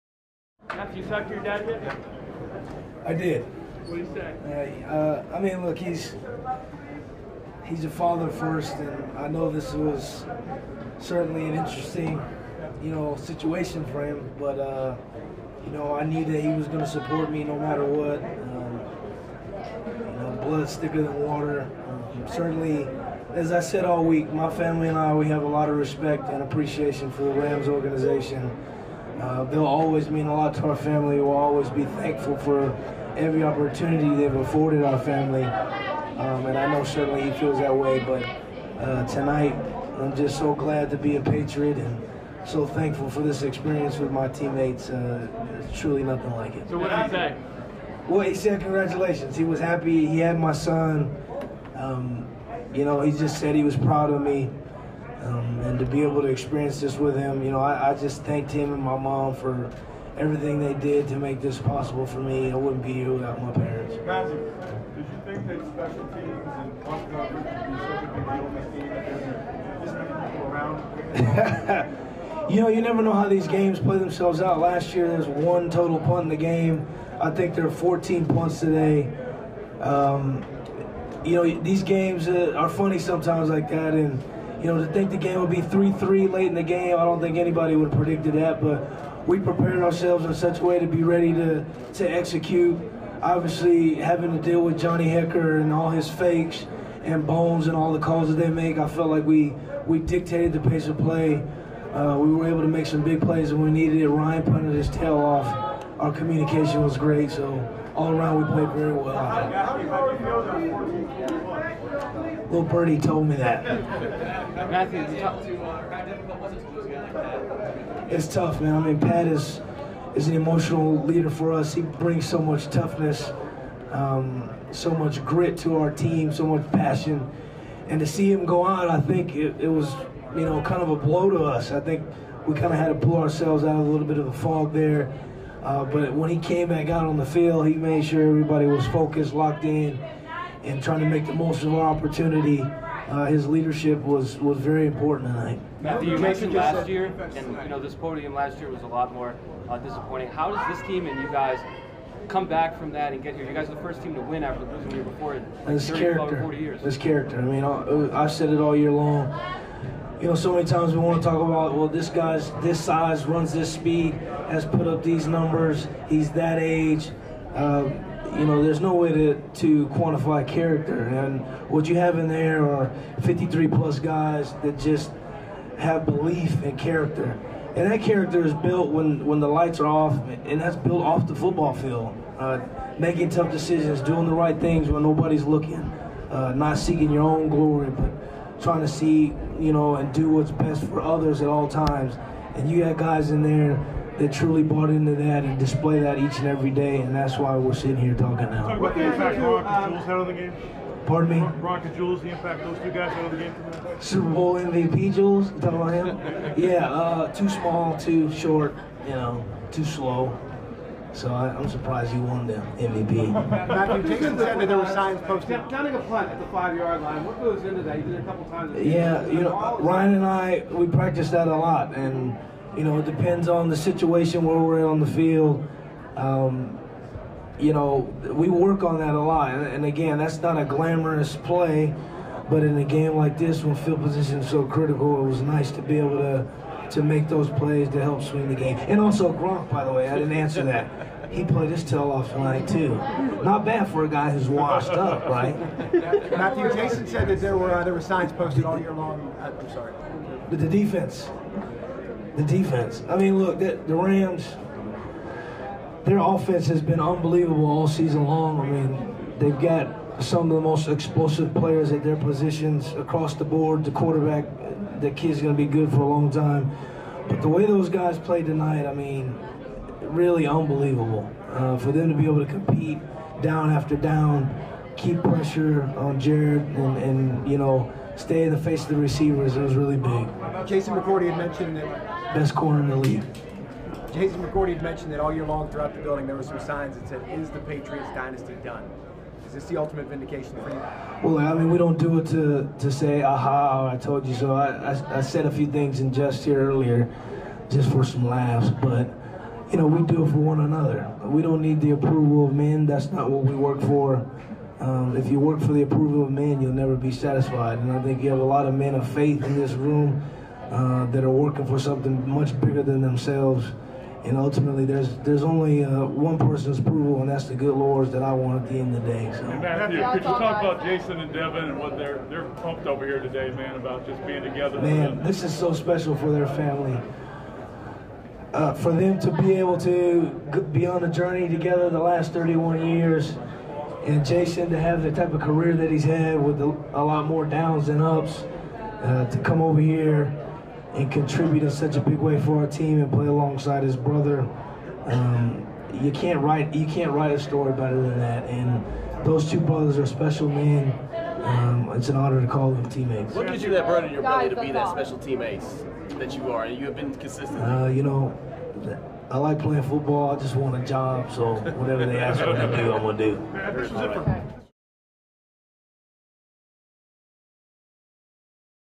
Matthew Slater after Super Bowl LIII win: "I'm just so glad to be a Patriot"
Matthew Slater addresses the media following the Patriots 13-3 win over the Los Angeles Rams in Super Bowl LIII on Sunday, February 3, 2019